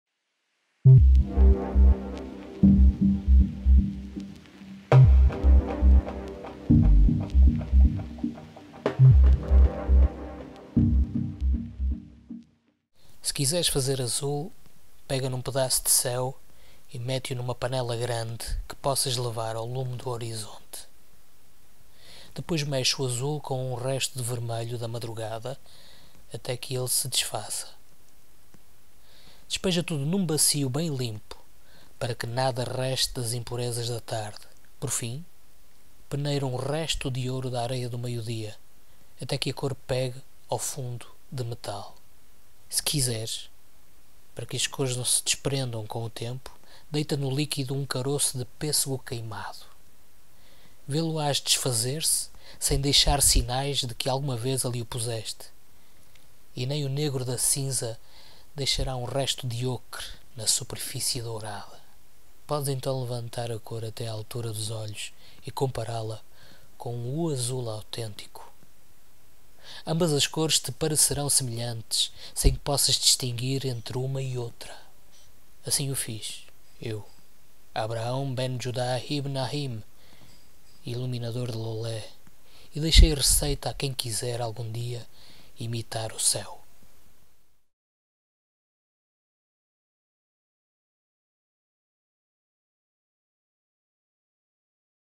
LEITURA PARA O COVID-19 ACHAR QUE JÁ MORREU - 33
Nestes dias de clausura, de Estado de Emergência, leituras de textos e poemas de poetas e escritores que ousaram verter no papel as letras do seu sonhar e do seu entendimento.

Texto: "Se Quiseres Fazer Azul" de Nuno Júdice, In "Meditação sobre ruínas, 1994".